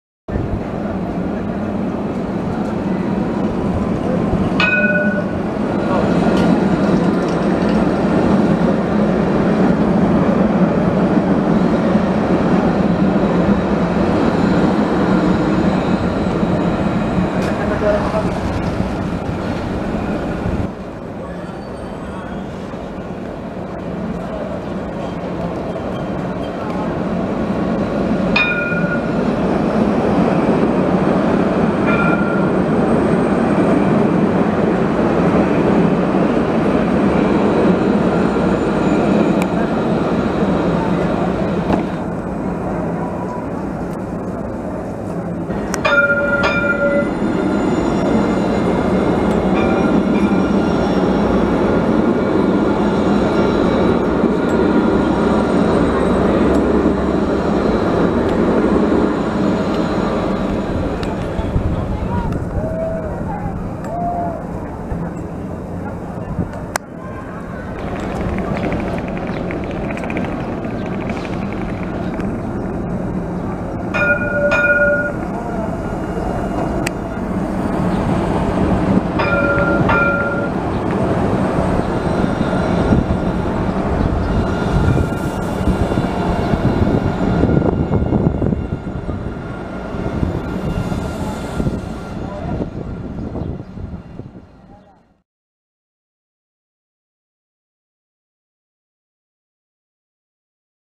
Israel - Jerusalem light rail - רכבת קלה - Tramway - Straßenbahn - Villamos - סיטיפס
The line is 13.9 kilometers (8.6 mi) long with 23 stops. Extensions to the red line are currently under construction to the northern suburb of Neve Yaakov and to Hadassah Ein Kerem Hospital to the southwest. Doubles set Citadis 302 trams (Jun 2017)